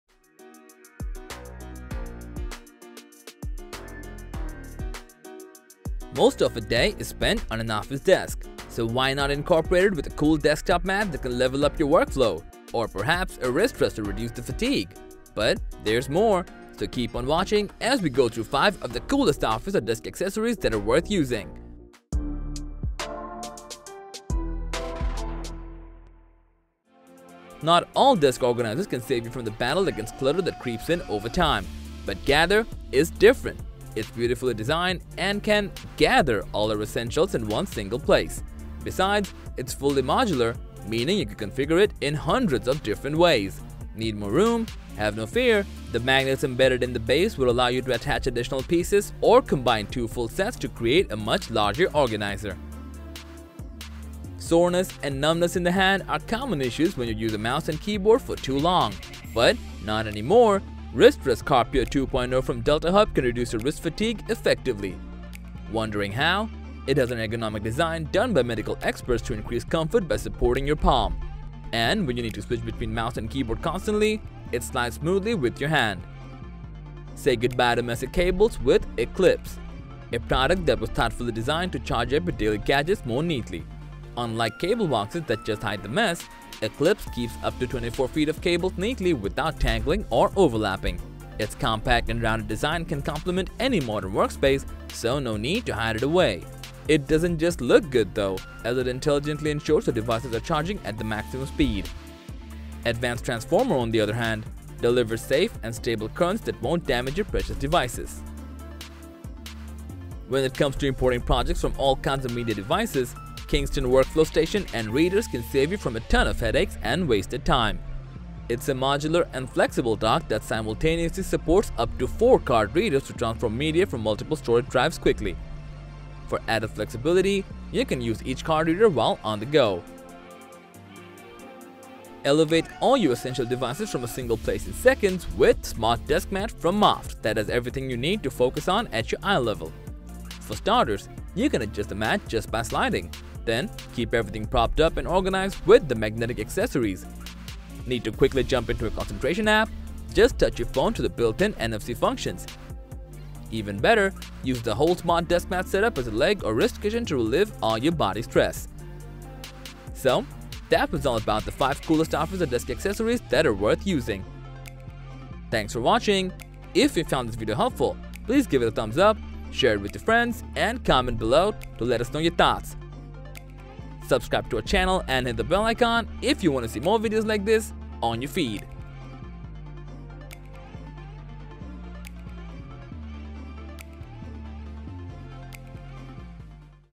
5 Coolest PC Accessories That Are Worth Using ▶ 4
Coolest PC Accessories can elevate how you work with your PC and enhance your productivity smoothly. These PC gadgets come in all shapes and sizes and offer limitless possibilities of what you can do with your setup. So here are the 5 Coolest PC Accessories That Are Worth Using.